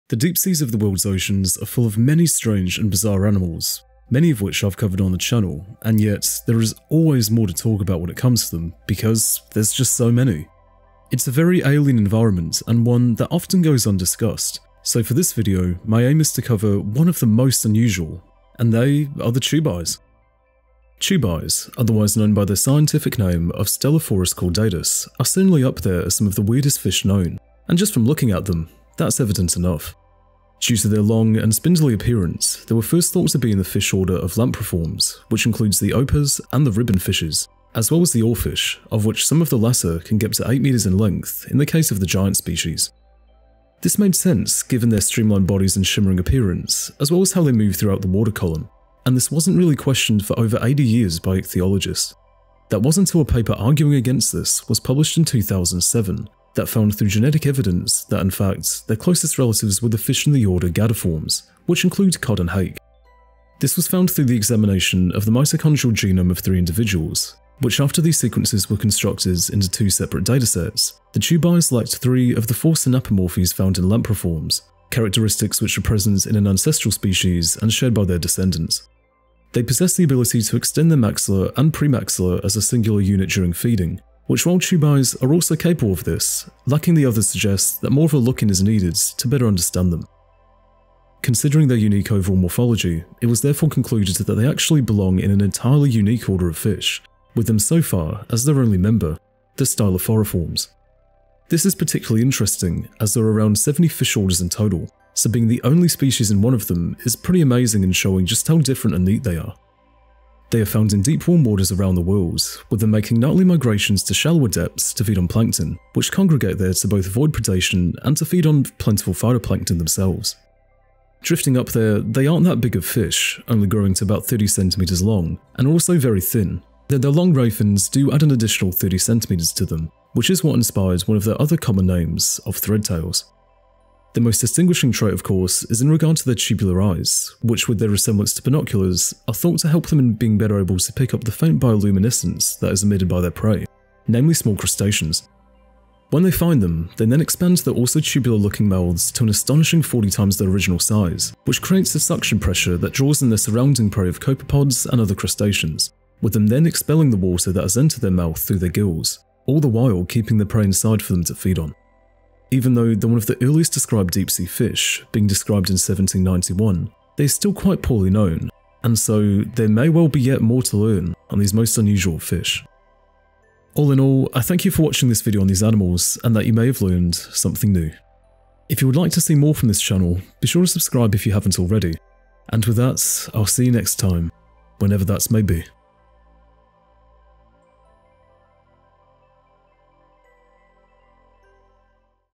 Tube-Eyes Explained | Nature's Weirdest Fish
The deep seas of the worlds oceans are filled with many bizarre animals, though of course, theres always more to talk about on them, because, well, there's just so many! These deep sea environments are very alien to us, and so they often get under-discussed, so the aim for this video is to talk about one of the most obscure of these animals, being the tube-eyes, fish that have very large, bug-like eyes, as well as mouths that distend to a crazy degree.